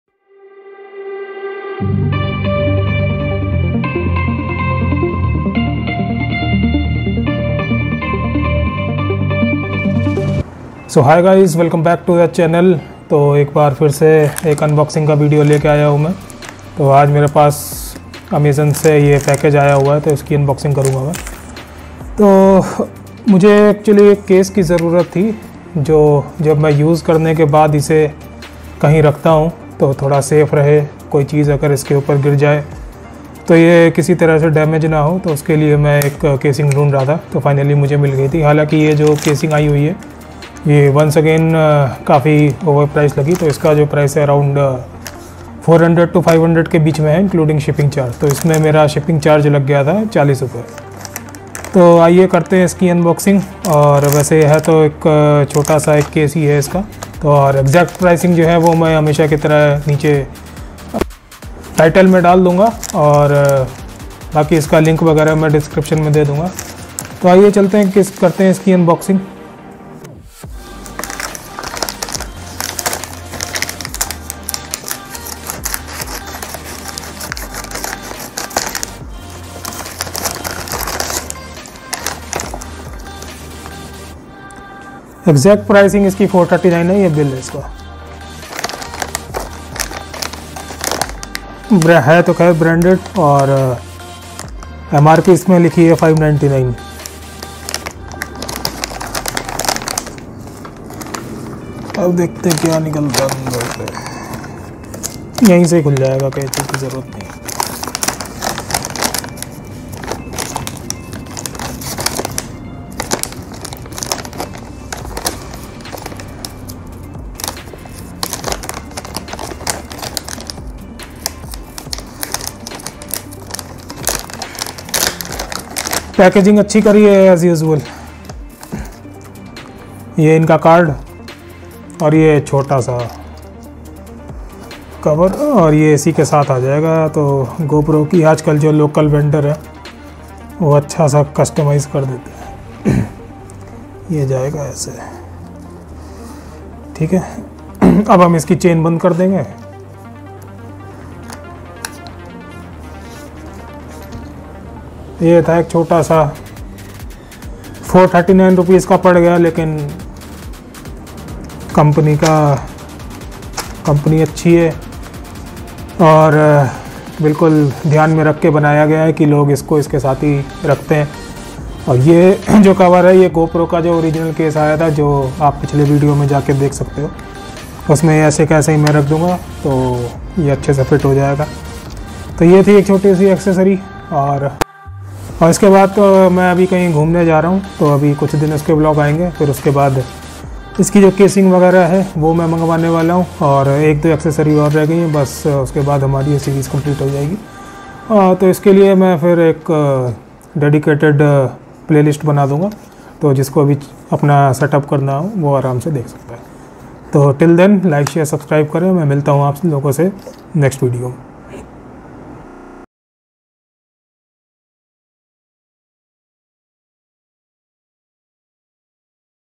Action Pro Portable Hard Carrying Case Bag Compatible With GoPro Hero 10 9 Black
In this video I have done unboxing of Action Pro Portable Hard Carrying Case Bag Compatible with GoPro Hero 10 9 Black bought for Rs 439